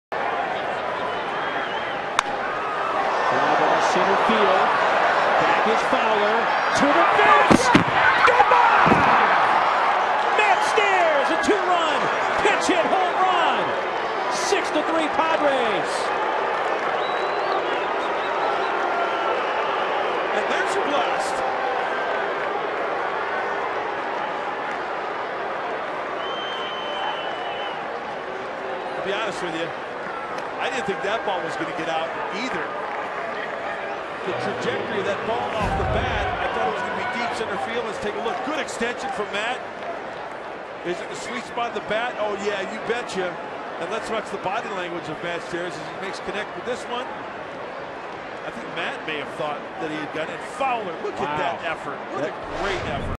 SD@COL: Stairs hits a pinch hit two-run shot to center
9/14/10: Matt Stairs hammers a pinch hit two-run home run to center field to give the Padres some breathing room in the 8th.